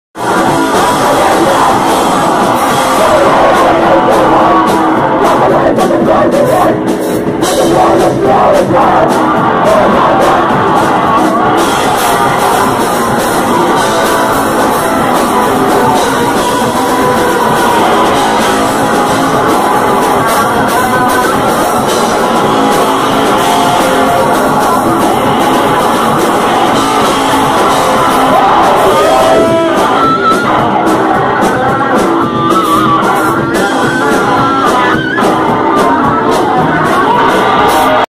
Lotus Fucker live @ Hellunião Ruidosa I - Fortaleza/Brazil - 15/10/2015
Small cut from Lotus Fucker show in Fortaleza/CE/Brazil. 2015.